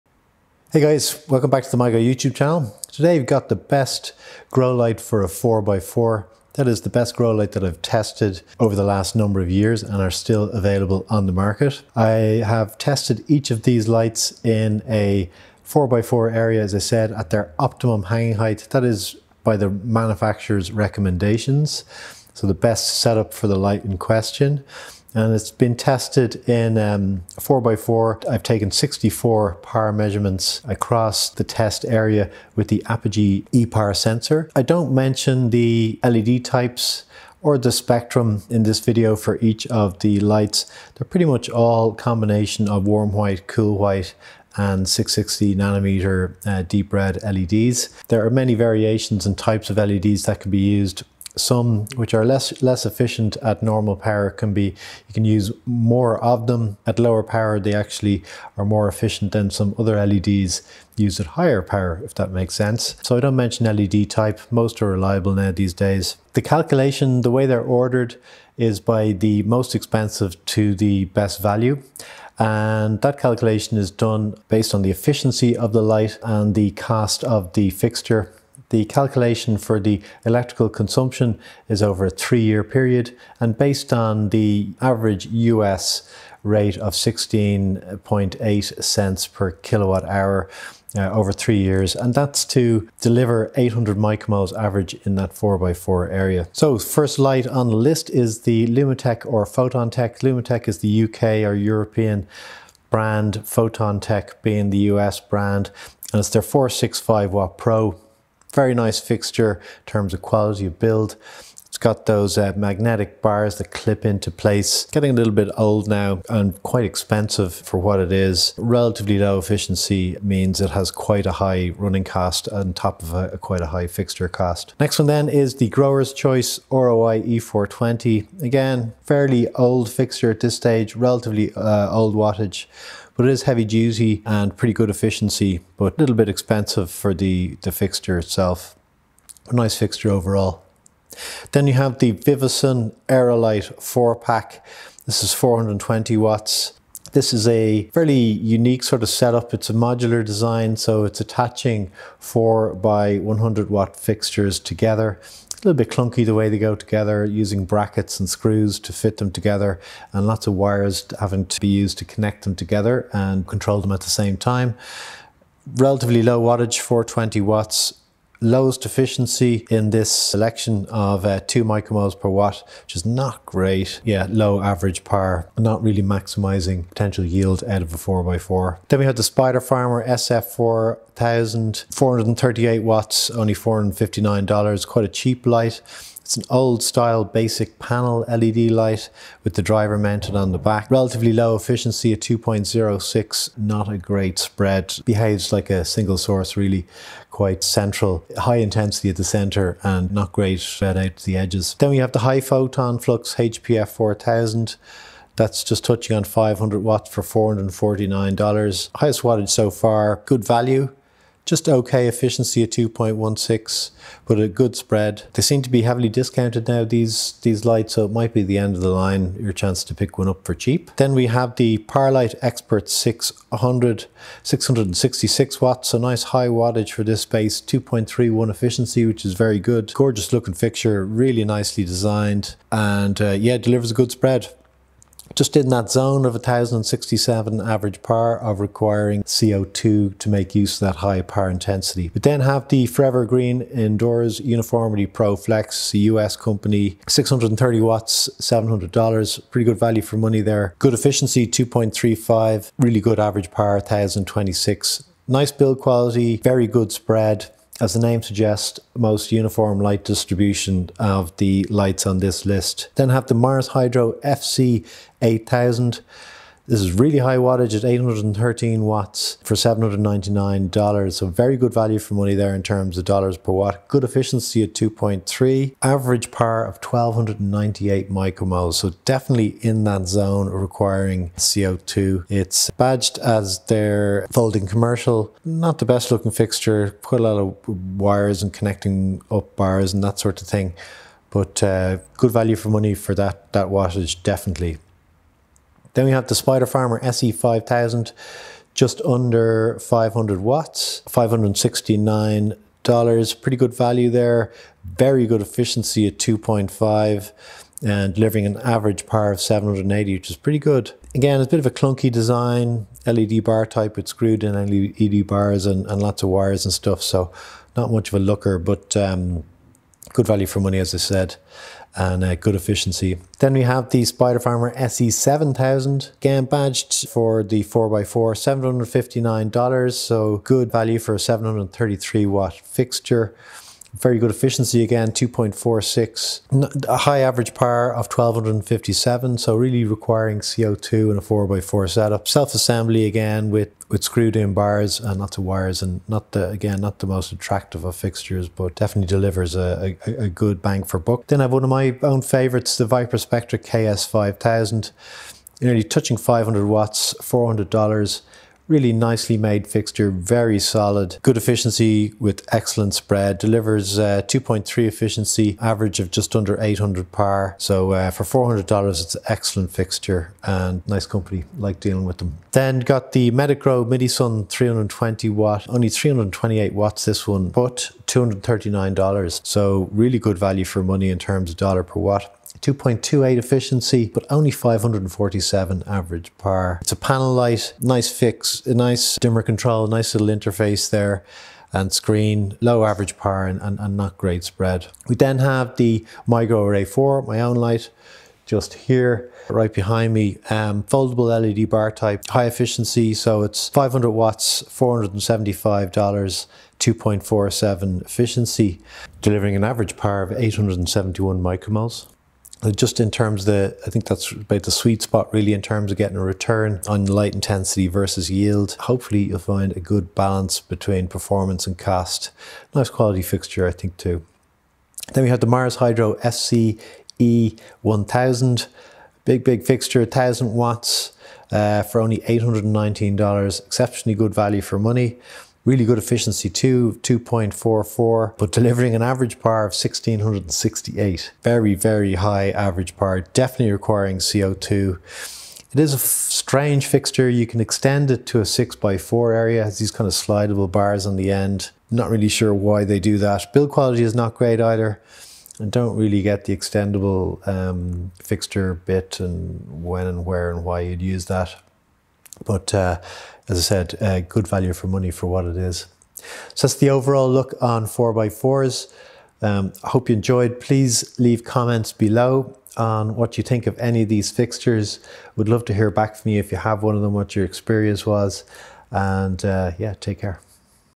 Best 4x4 grow light 2024
I review the best grow lights I have tested for a 4ft x 4ft or 120cm x 120cm grow space. These are the best grow lights from 400 watts to 600 watts (with some as high as 800 watts) I have tested and are available on the market today.

The calculation for 3 year cost is based on delivering 800 µmols average PAR to the plant canopy in a 4ft x 4ft area. The cost includes the price of the fixture and the cost of electricity at the average US rate of 16.8C per KW hour  run for 3 years at 12 hours per day.